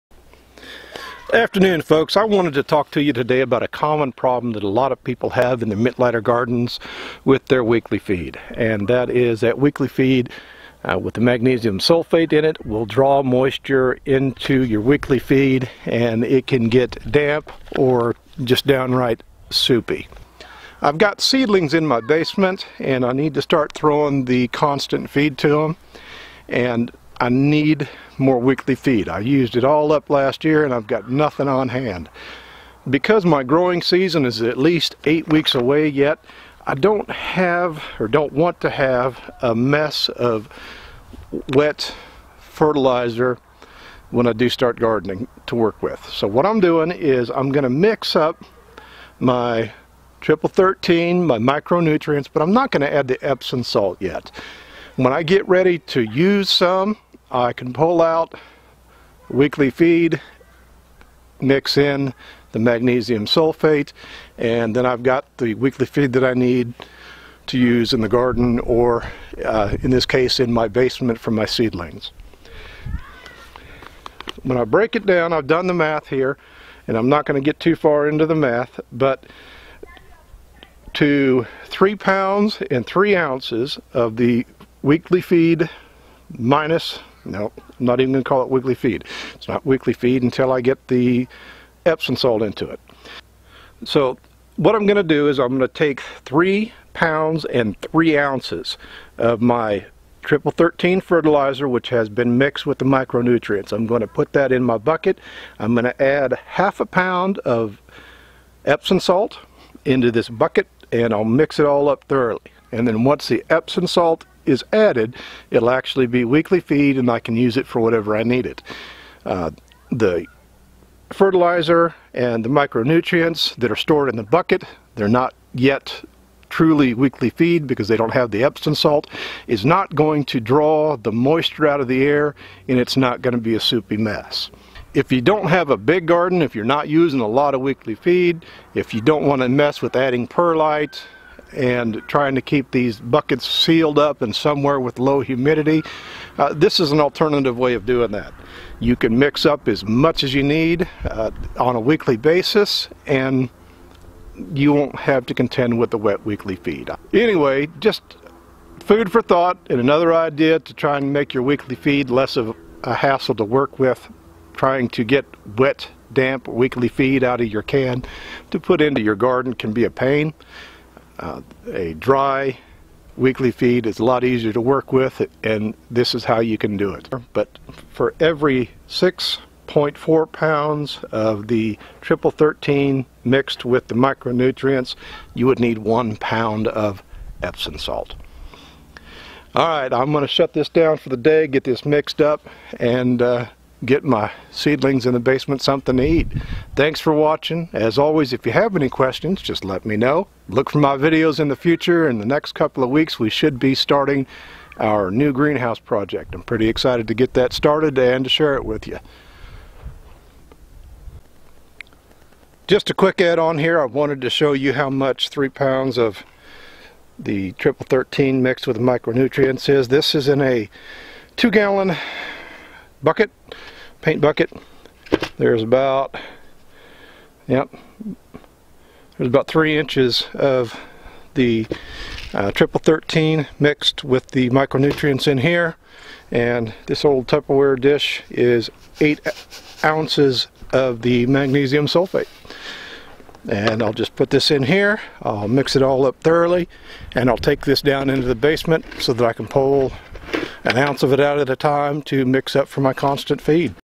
How to keep your Mittleider weekly feed fertilizer dry
The Mittleider weekly feed that becomes wet can be a hassle.  In this video I will show you an easy and no cost way on how to keep your Mittleider weekly feed fertilizer dry. If you have a small garden a batch of weekly feed will be wet like soup before you use it all.  To avoid the damp weekly I show you how I mix small batches.  Until you add the Epsom salt it won't draw moisture from the air.  This is a quick video explaining how and why I mix in small batches to avoid the wet weekly feed.